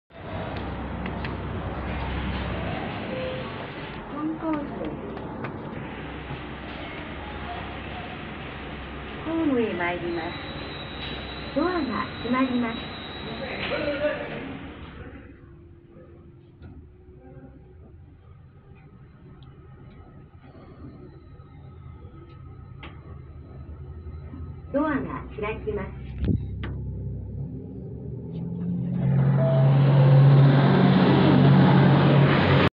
三菱エレベーター 更新前
後免駅にあります。まだ未更新のままで運転しています。機種はエレパックです。方式は機械室あり油圧式です。
メーカー:三菱
設置年:2001年
定員:11名
積載:750kg
キャンセル機能:なし
アナウンス:あり
戸閉ブザー:なし
到着ベル音:あり (アナウンスの中に組み込まれている電子音チャイム)
ボタン確認音:なし
停止階:1.2
停止可能階:1.2